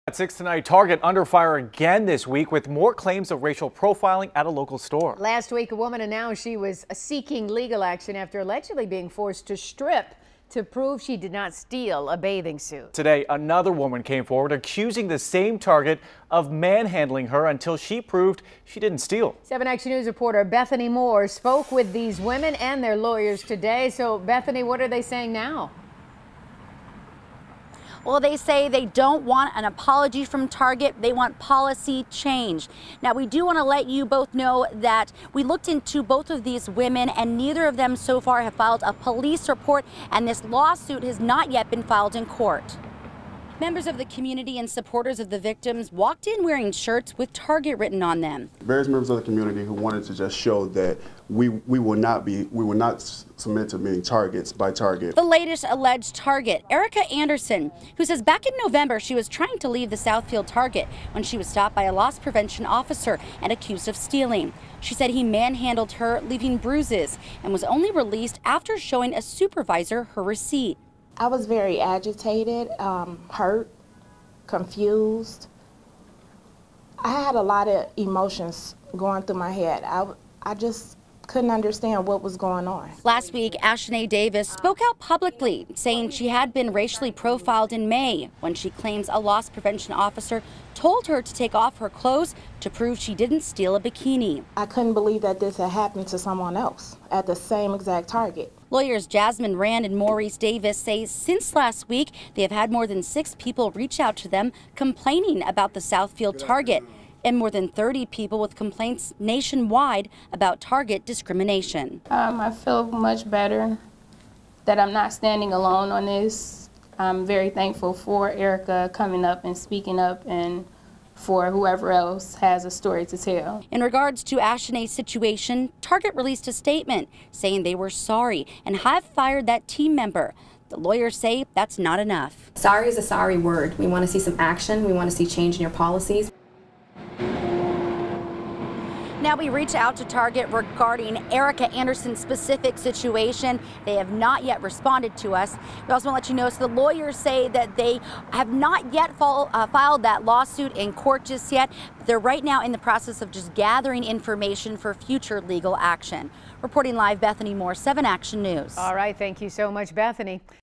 Second woman alleges she was racially profiled at Southfield Target store
Another woman has come forward alleging that she was racially profiled at a Southfield Target store.  In a news release, the attorneys representing 23-year-old Ashanae Davis who first claimed that two Target loss prevention officers confronted her on May 22, accusing her of stealing a bikini from the store.